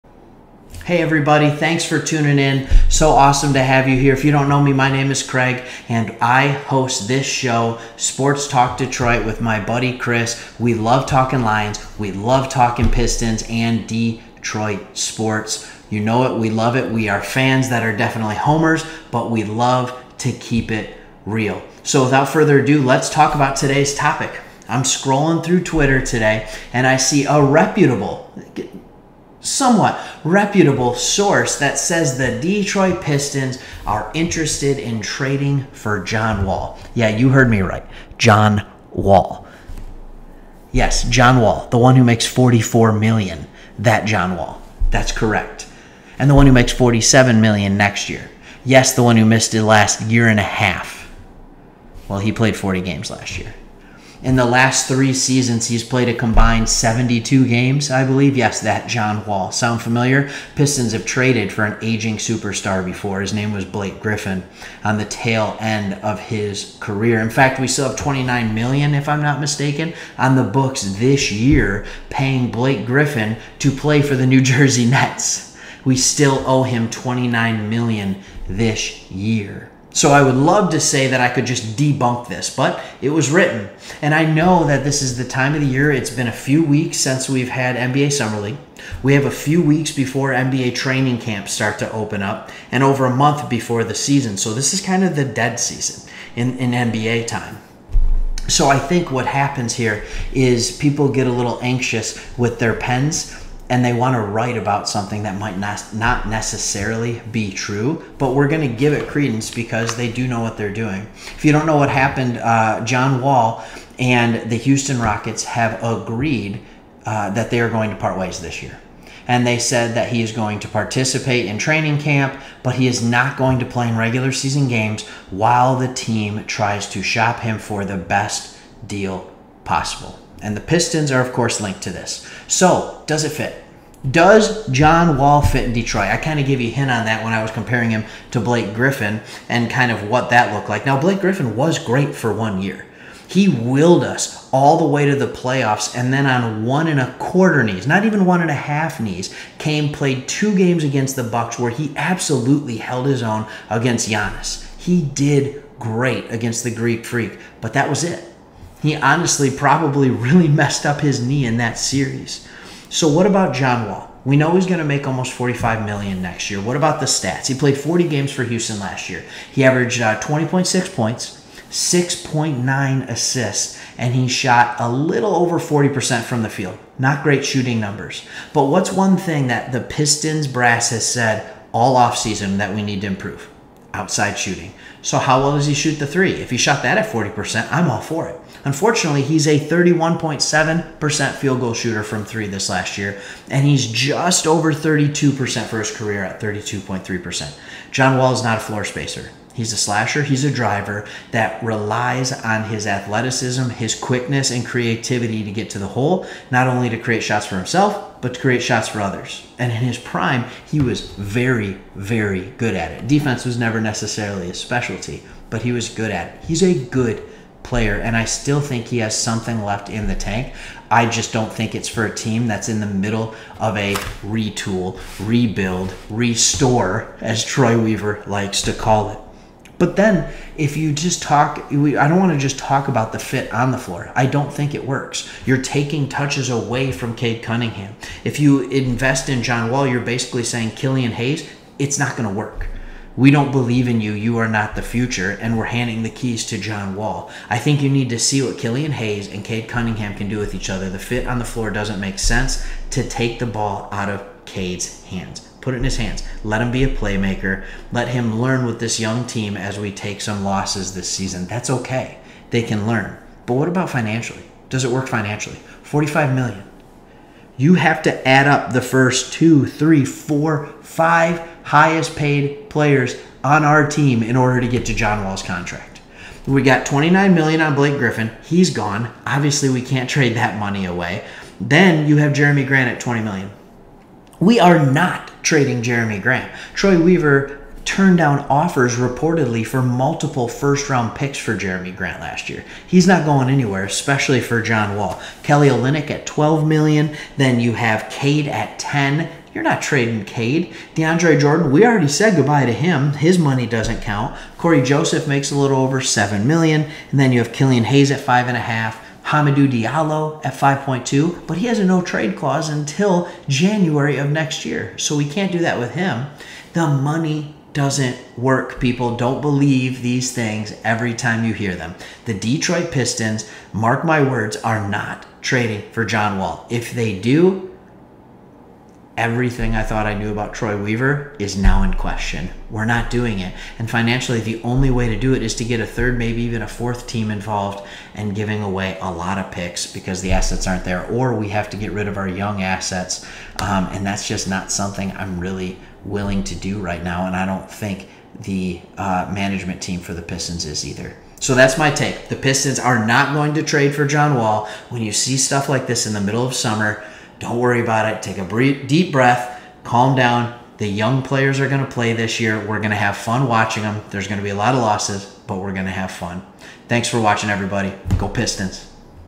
John Wall Trade Rumors! Reports say the Detroit Pistons are interested
In this video Craig talks about the rumor that the Pistons are interested in John Wall.  Rumors have been swirling that a few teams including the Pistons are looking at trading for the aging star and that John Wall and the Houston Rockets have agreed to attempt a trade.

In this video we talk about what a trade could look like, the potential fit with John Wall on the Pistons, and the Salary Cap implications of the trade.